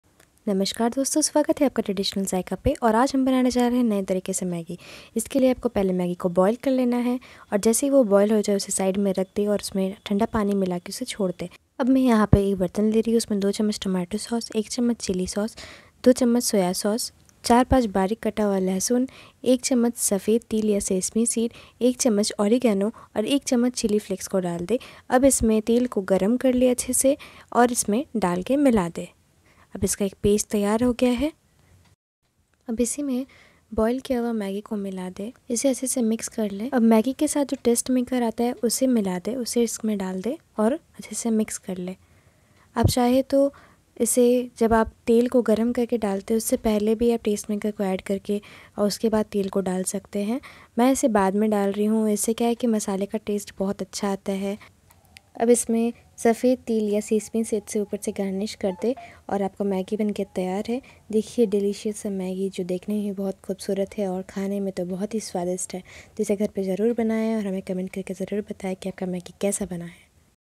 New Style Maggi Recipes|| Korean Style Maggie
Hey Foodies here I am with a new recipe ..
Korean Style Maggie Recipe 😋

Ingredients:-
Maggie
oil
sesame seeds
oregano
chilli flakes
tomato sauce
chilli sauce
soya sauce